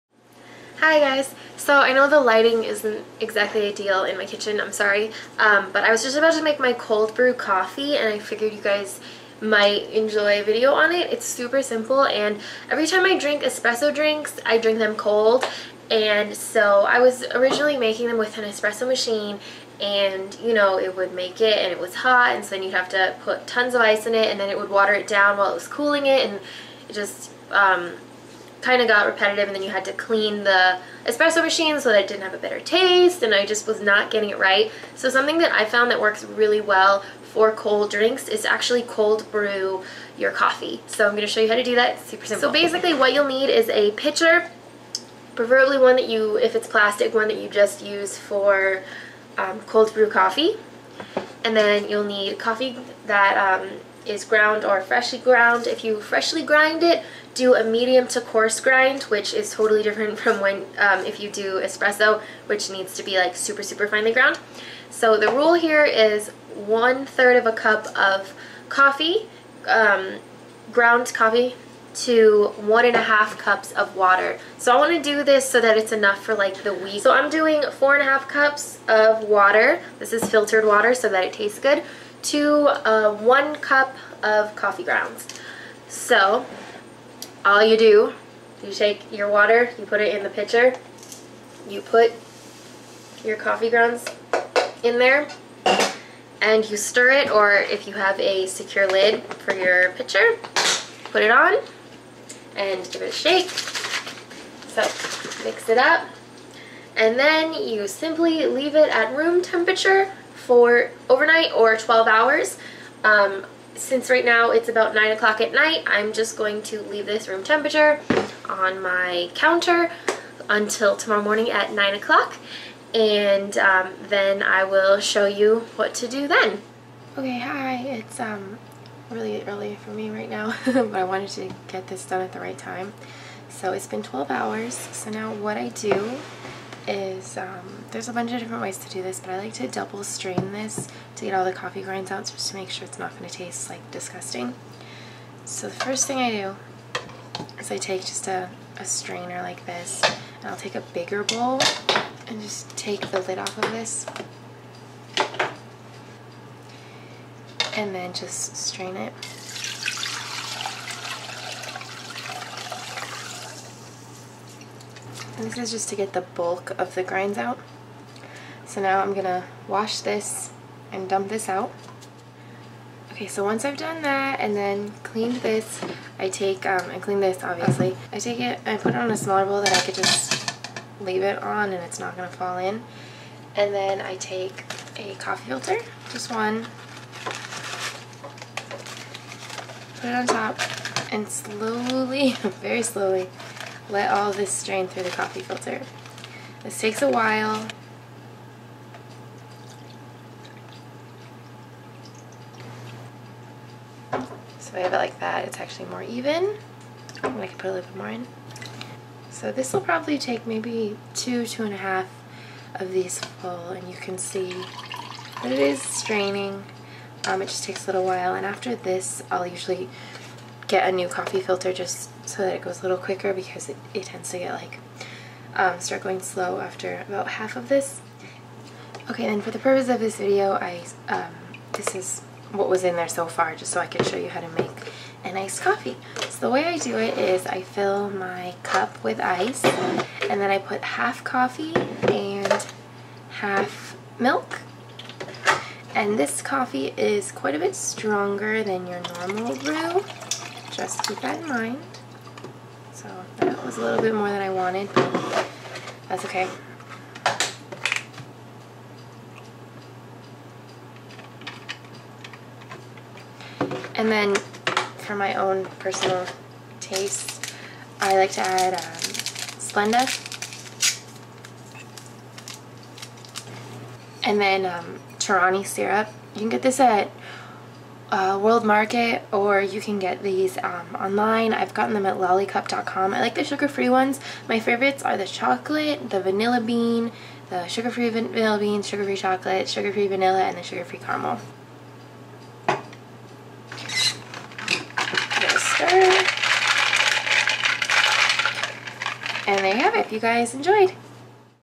How to Make Cold Brew Coffee
rule- 1/3cup coffee gounds for every 1 1/2 cups water

stir together in pitcher, leave untouched overnight or for 12 hours, double strain, enjoy! Keep it in your fridge so it lasts longer :)